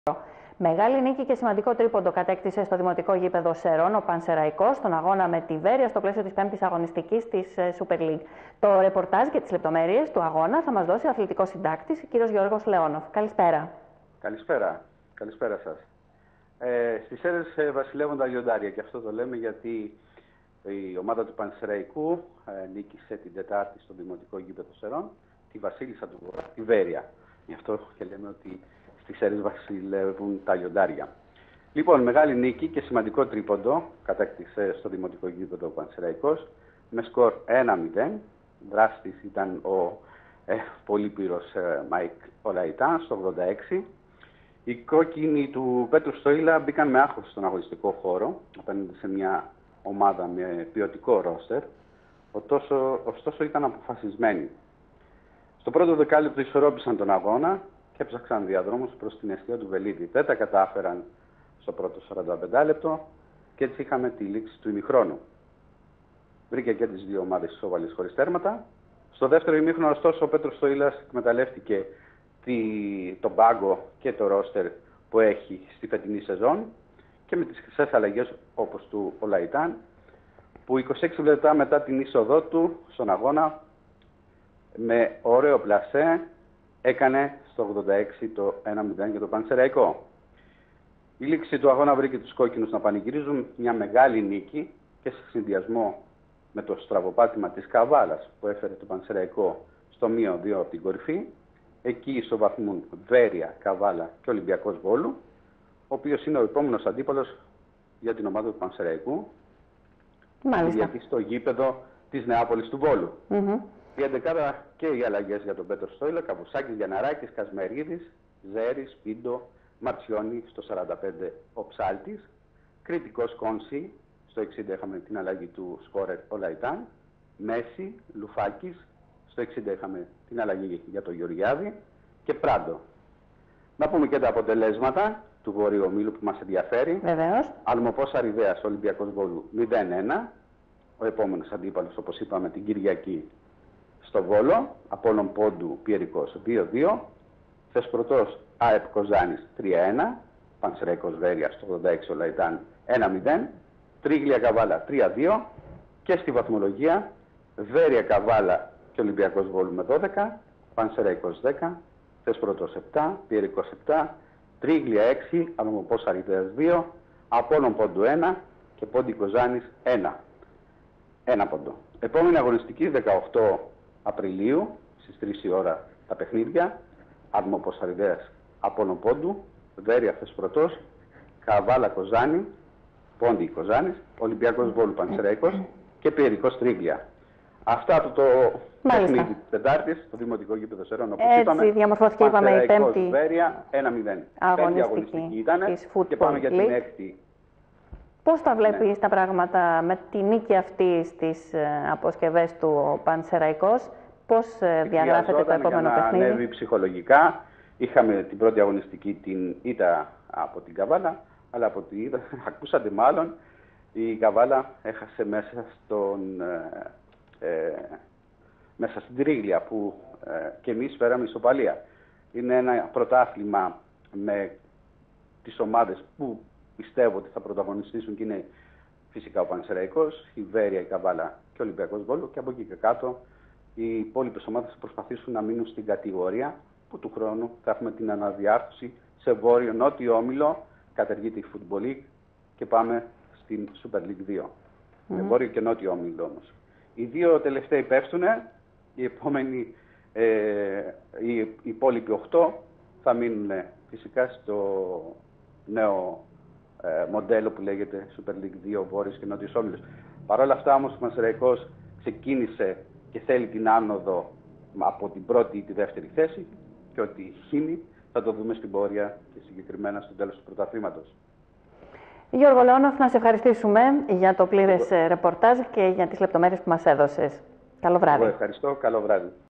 Στις Σέρρες "βασιλεύουν" τα "λιοντάρια"
Μεγάλη νίκη και σημαντικό ''τρίποντο'', κατέκτησε στο Δημοτικό Γήπεδο Σερρών ο Πανσερραϊκός, με σκορ 1-0 , και ''δράστη'' τον πολύπειρο Μάικλ Ολαϊτάν, στο 86'.